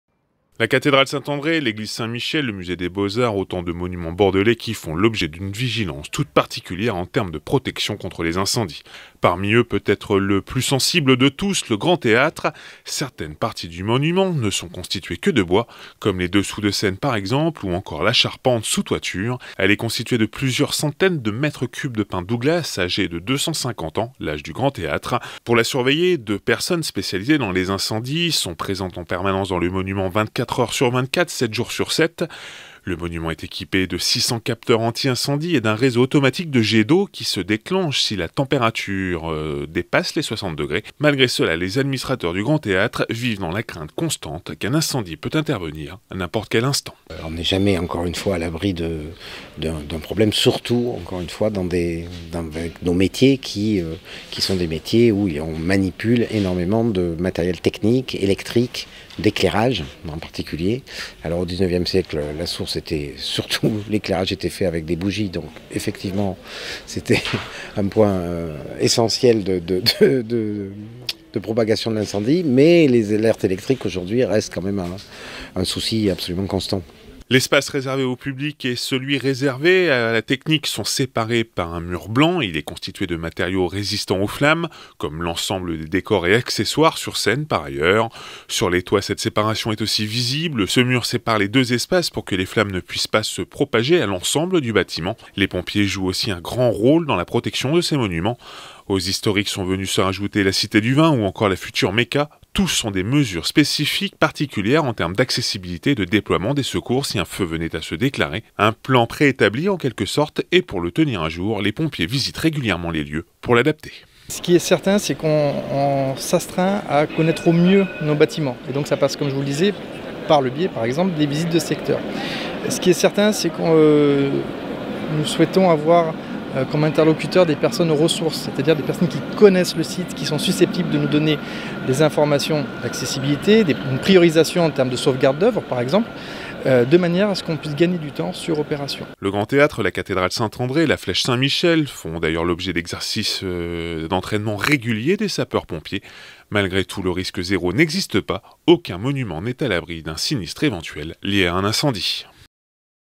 Les mesures de protection des monuments bordelais contre les incendies...
Au lendemain du drame de Notre-Dame de Paris, se pose la question de la protection des monuments emblématiques de la ville de Bordeaux contre les incendies. Bénéficient-ils de mesures particulières ? La réponse en images !